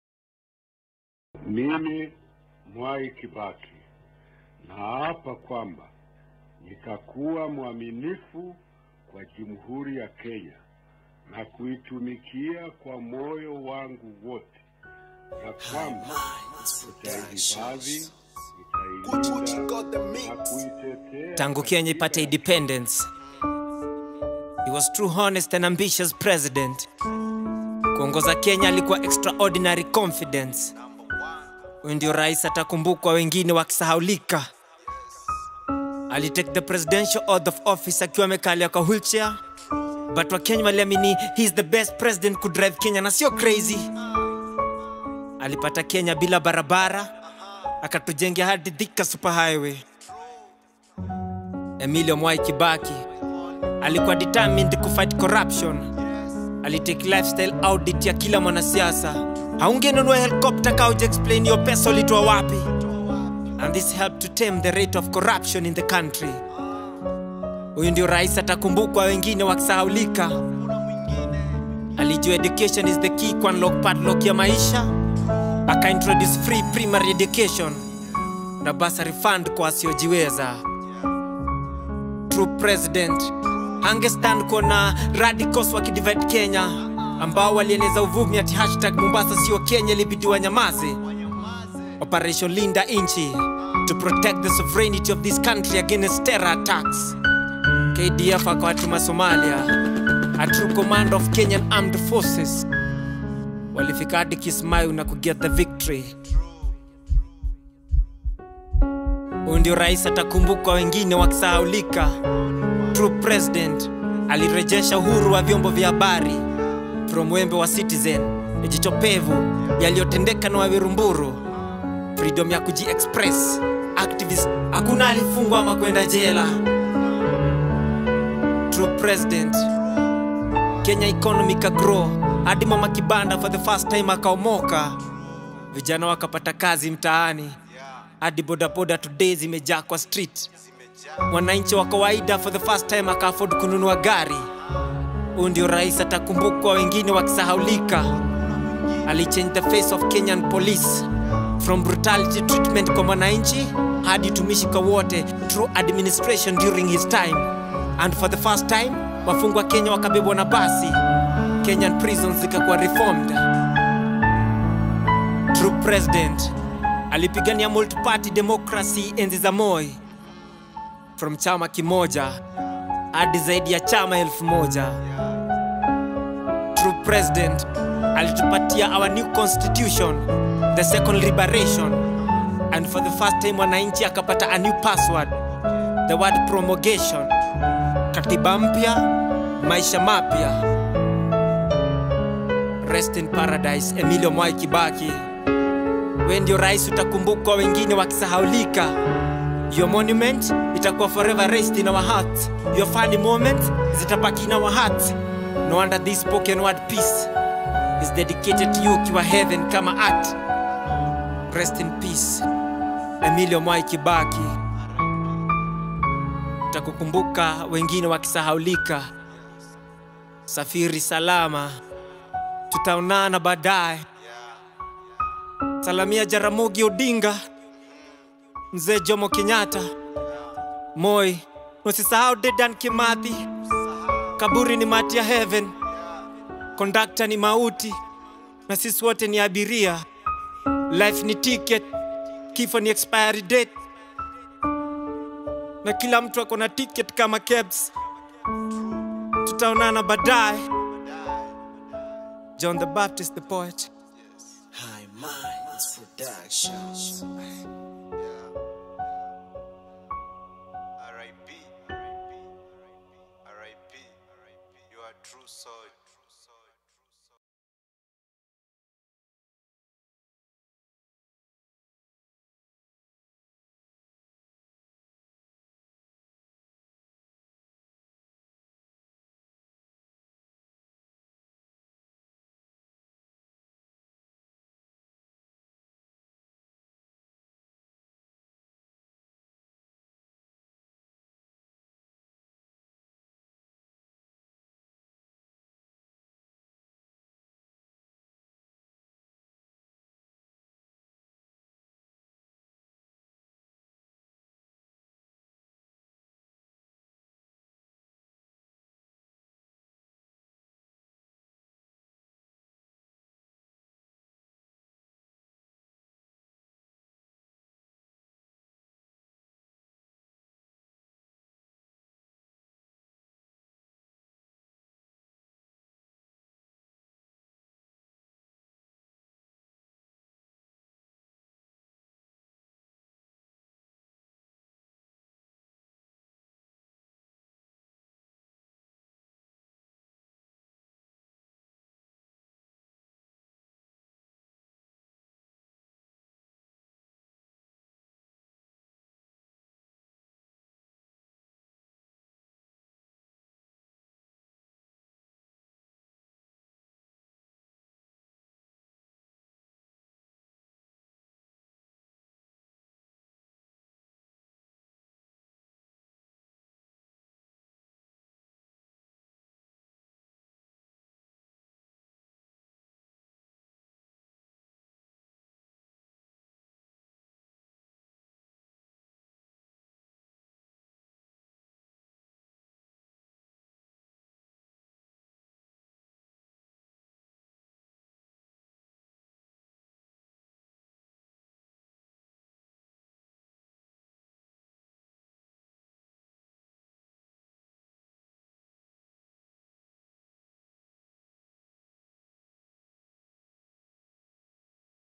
MWAI KIBAKI TRIBUTE/SPOKEN WORD poetry/POET JOHN THE BAPTIST KENYA official video made by Youcut app
video courtesy of different Media station's